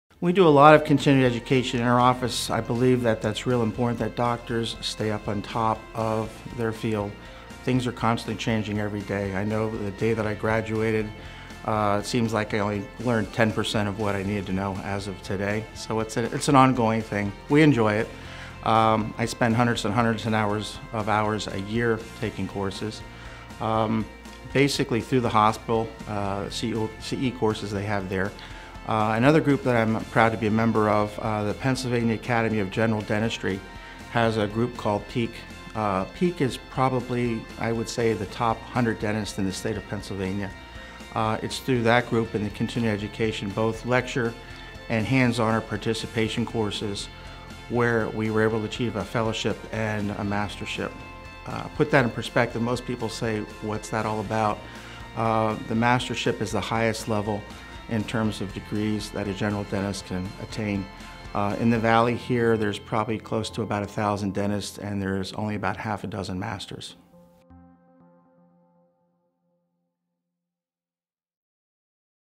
Dr. Thomas Lambert on Continuing Education in Dentistry
Dr. Lambert aims to learn more on all about dentistry to give his patients good dental service.

Follow them on:

and
Google+